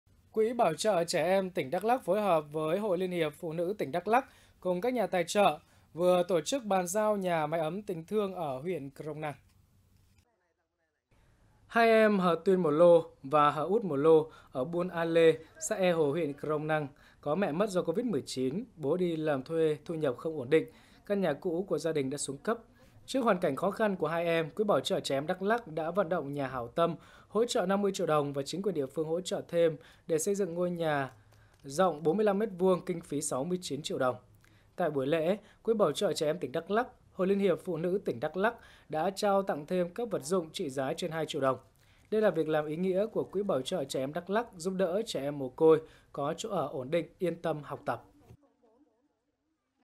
Ban giao nhà mái ấm tình thương cho trẻ em mồ côi huyện Krông Năng| Truyền hình Đắk Lắk
Ban giao nhà mái ấm tình thương cho trẻ em mồ côi huyện Krông Năng#thdaklak

ĐÀI PHÁT THANH TRUYỀN HÌNH ĐẮK LẮK